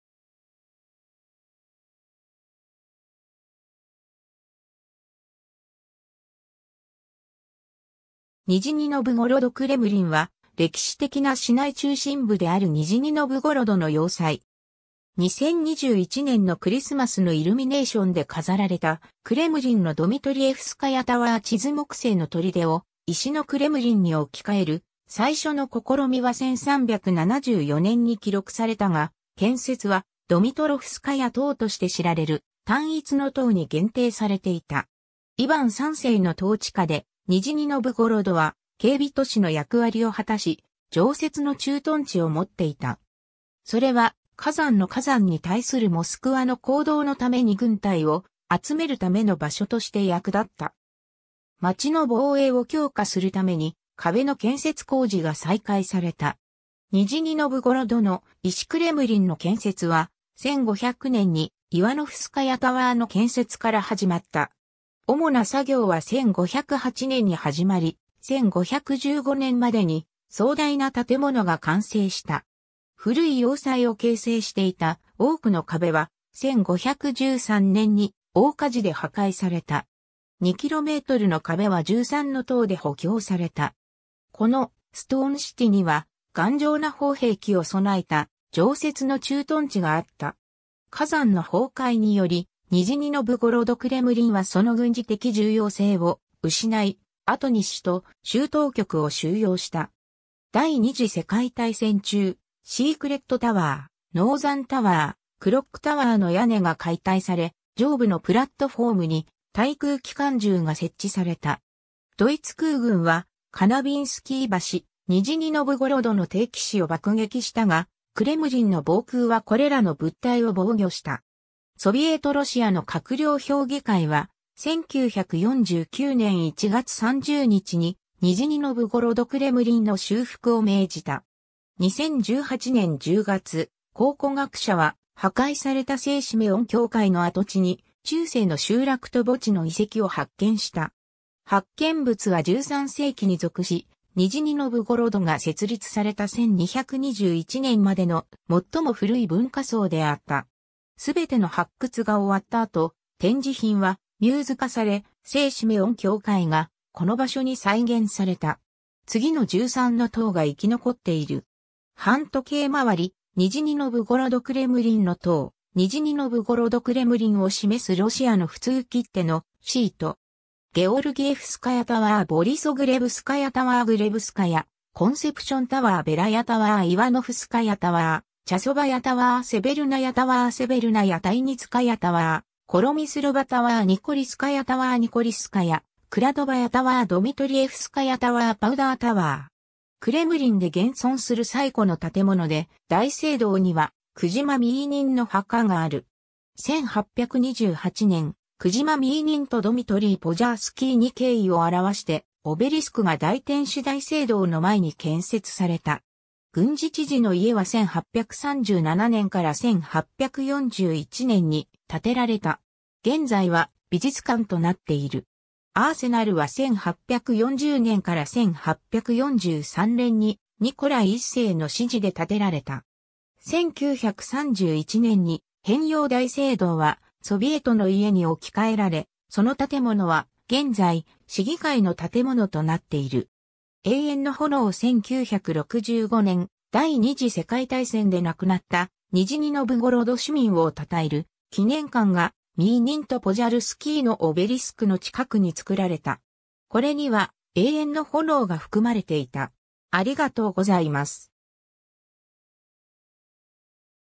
ニジニ・ノヴゴロドクレムリン
ソ連・ロシアの要塞
ニジニ・ノヴゴロド
クジマ・ミーニン
ドミートリー・ポジャールスキー
ニジニ・ノヴゴロドクレムリン（ロシア語: Нижегородский кремль ）は、歴史的な市内中心部であるニジニ・ノヴゴロドの要塞。
 2021年のクリスマスのイルミネーションで飾られたクレムリンのドミトリエフスカヤタワー 地図 木製の砦を石のクレムリンに置き換える最初の試みは1374年に記録されたが、建設はドミトロフスカヤ塔として知られる単一の塔に限定されていた（これは残っていない）。
イヴァン3世の統治下で、ニジニノヴゴロドは警備都市の役割を果たし、常設の駐屯地を持っていた。
それはカザンのカザンに対するモスクワの行動のために軍隊を集めるための場所として役立った。
街の防衛を強化するために、壁の建設工事が再開された。
 ニジニノヴゴロドの石クレムリンの建設は、1500年にイワノフスカヤタワーの建設から始まった。
 主な作業は1508年に始まり、1515年までに壮大な建物が完成した。
古い要塞を形成していたオークの壁は、1513年に大火事で破壊された。
2kmの壁は13の塔で補強された（そのうちの1つであるザチャチエフスカヤはヴォルガ川の岸にあった。保存されていなかったが、2012年に再建された）。
 この「ストーンシティ」には、頑丈な砲兵器を備えた常設の駐屯地があった。
 カザンの崩壊により、ニジニノヴゴロドクレムリンはその軍事的重要性を失い、後に市と州当局を収容した。
 第二次世界大戦中、シークレットタワー、ノーザンタワー、クロックタワーの屋根が解体され、上部のプラットフォームに対空機関銃が設置された。
 ドイツ空軍はカナヴィンスキー橋、ニジニ・ノヴゴロドの定期市を爆撃したが、クレムリンの防空はこれらの物体を防御した。
 ソビエトロシアの閣僚評議会は、1949年1月30日に、ニジニノヴゴロドクレムリンの修復を命じた。
 2018年10月、考古学者は、破壊された聖シメオン教会の跡地に中世の集落と墓地の遺跡を発見した。
発見物は13世紀に属し、ニジニノヴゴロドが設立された1221年までの最も古い文化層であった。
 すべての発掘が終わった後、展示品はミューズ化され、聖シメオン教会がこの場所に再現された。
 次の13の塔が生き残っている。
反時計回り： ニジニノヴゴロドクレムリンの塔 ニジニノヴゴロドクレムリンを示すロシアの普通切手のシート。
 ゲオルギエフスカヤタワー（ロシア語: Георгиевская башня 'セントジョージタワー'） ボリソグレブスカヤタワー（ロシア語: Борисоглебская башня '聖ボリスとグレブ塔'; 18世紀に地滑りにより破壊され、1972年に再建された）グレブスカヤ コンセプションタワー（ロシア語: Зачатская башня 'コンペプションタワー'; 18世紀に地滑りにより破壊され、2012年に再建された） ベラヤタワー（ロシア語: Белая башня 'ホワイトタワー' ） イワノフスカヤタワー（ロシア語: Ивановская башня 'セントジョンズタワー'） チャソヴァヤタワー（ロシア語: Часовая башня '時計台' ） セヴェルナヤタワー（ロシア語: Северная башня 'ノーザンタワー'）セヴェルナヤ タイニツカヤタワー（ロシア語: Тайницкая башня '秘密の塔' ） コロミスロヴァタワー（ロシア語: Коромыслова башня 'ヨークタワー'） ニコリスカヤタワー（ロシア語: Никольская башня '聖ニコラスタワー'）ニコリスカヤ クラドヴァヤタワー（ロシア語: Кладовая башня 'パントリータワー'） ドミトリエフスカヤタワー（ロシア語: Дмитриевская башня 'デメトリウスタワー'） パウダータワー（ロシア語: Пороховая башня 'パウダータワー' ミーニンとポジャルスキーのオベリスクと大天使聖ミカエル大聖堂 クレムリンには多くの教会があったが、唯一の生存は、16世紀半ばまでに建てられ、1628〜1631年に再建された大天使ミカエル大聖堂（「大天使大聖堂」）。
クレムリンで現存する最古の建物で、大聖堂にはクジマ・ミーニンの墓がある。
 1828年、クジマ・ミーニンとドミトリー・ポジャースキーに敬意を表してオベリスクが大...